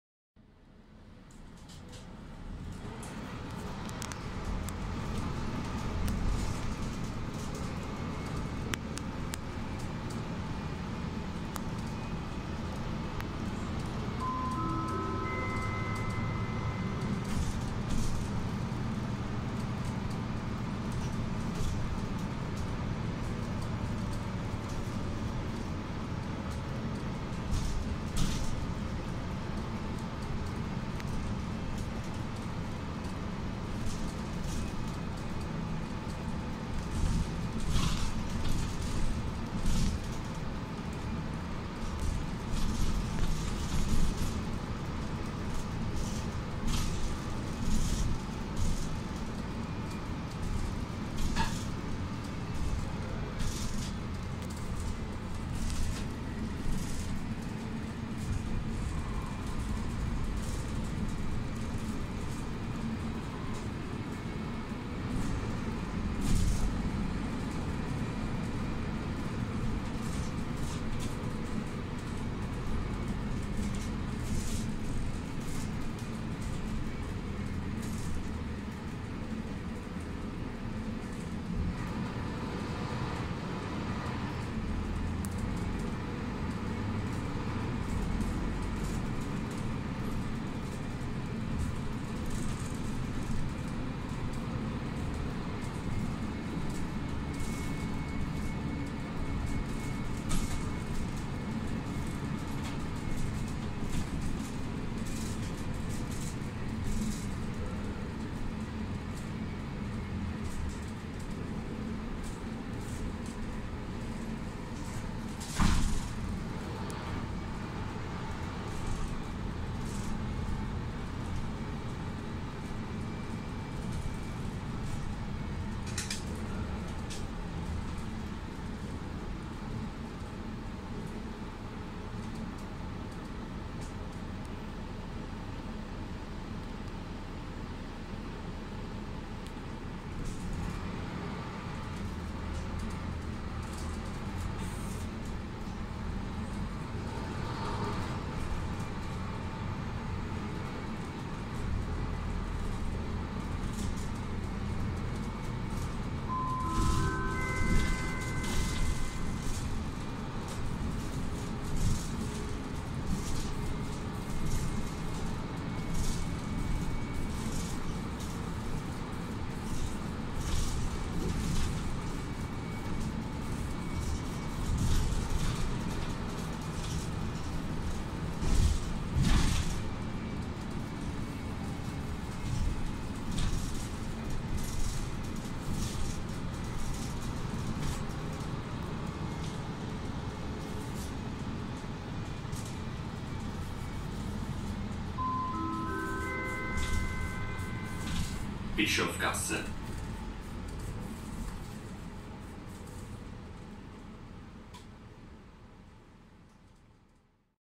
17A Oberlaa-Bischofgasse (Autobus Wien)
Mitfahrt in einem Mercedes Citaro 2 von Gschwindl von der Station Oberlaa (Laaer-Berg-Straße) über die seit 1. Juli 2019 bestehende Streckenführung Friedhofstraße - Kästenbaumgasse - Oberlaaer Straße zur Hst. Bischofgasse.